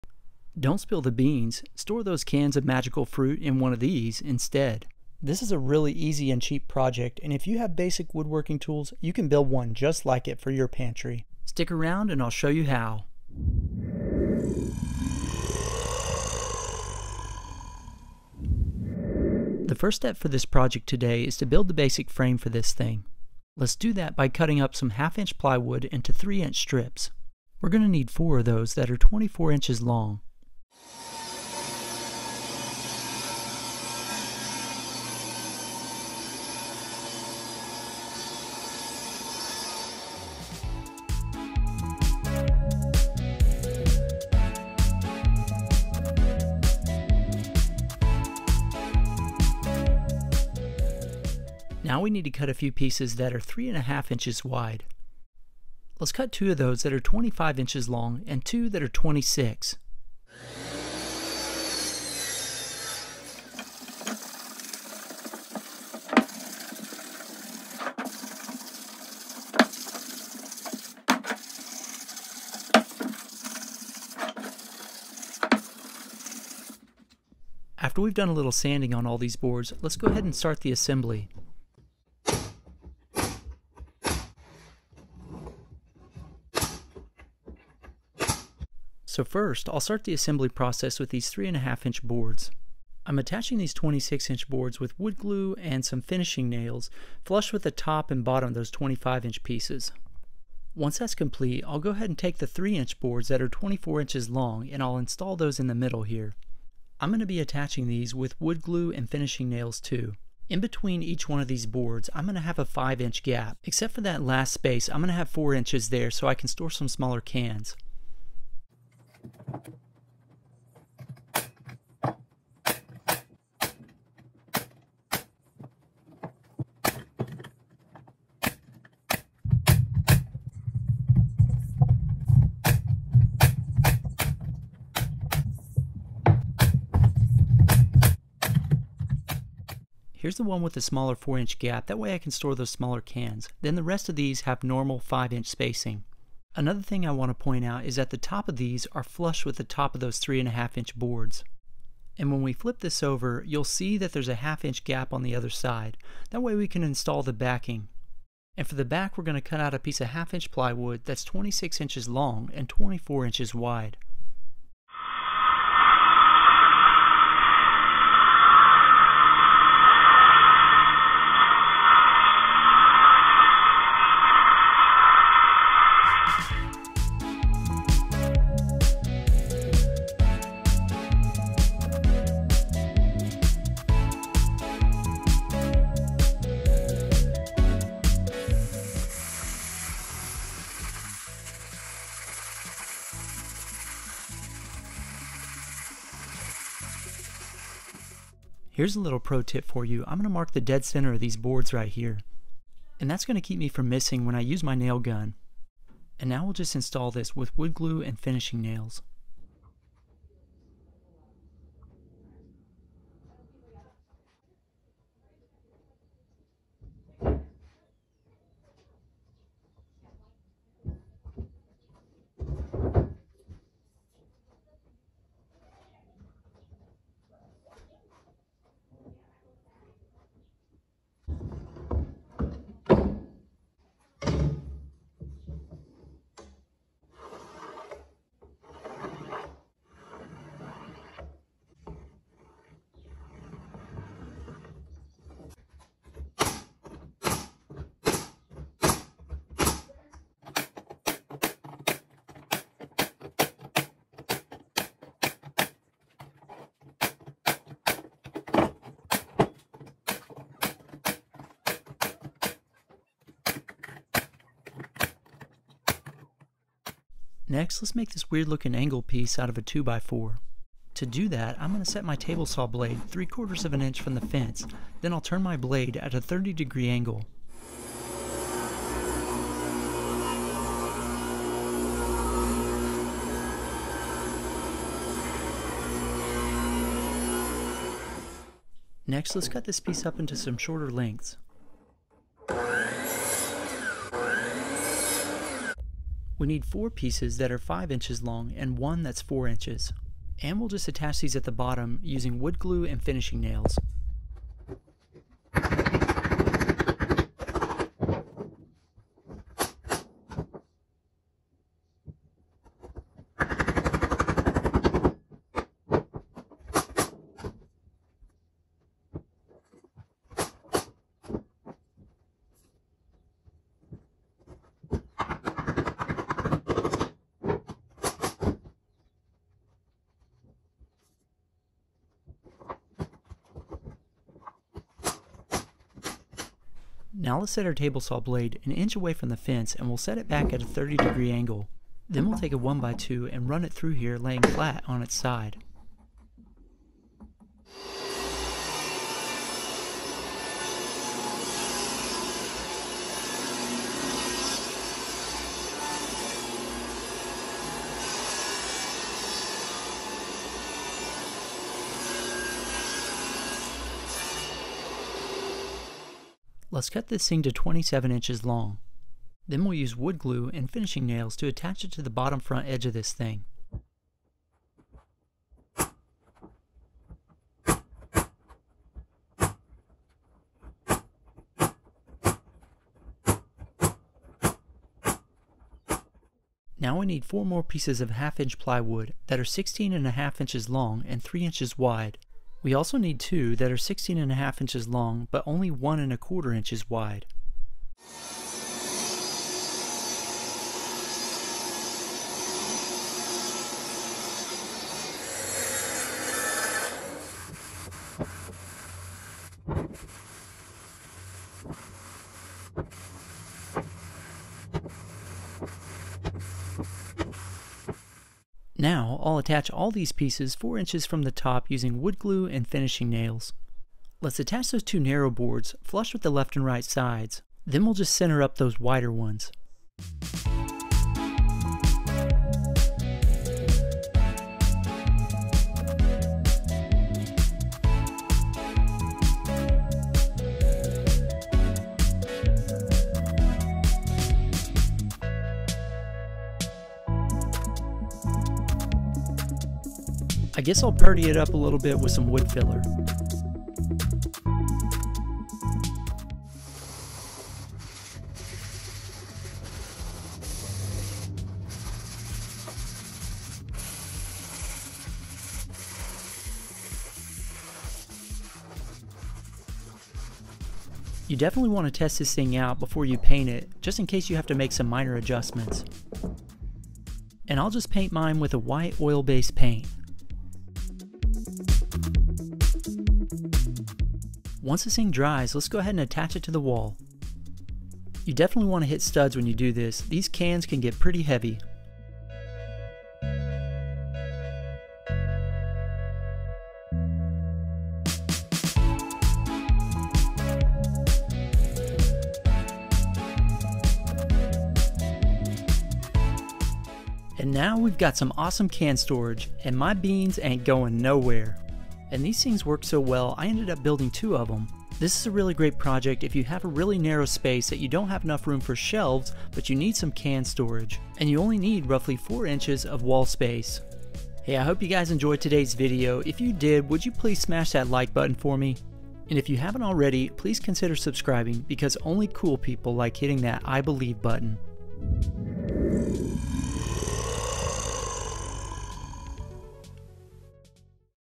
DIY Canned Food Storage Rack (Easy)
How to build a canned food storage rack.  This video shows all the steps to build a storage rack for your canned food.  This canned food rack is very cheap to build and you don’t need any fancy tools to build it.  Canned food can be heavy and take up a lot of space.  This cool storage idea solves that problem.

Material list:

2x4 1pc at 4ft
1x2 1pc at 4ft
4x4 sheet of 1/2” plywood 1pc

18ga brad nailer (I recommend the set)

Sander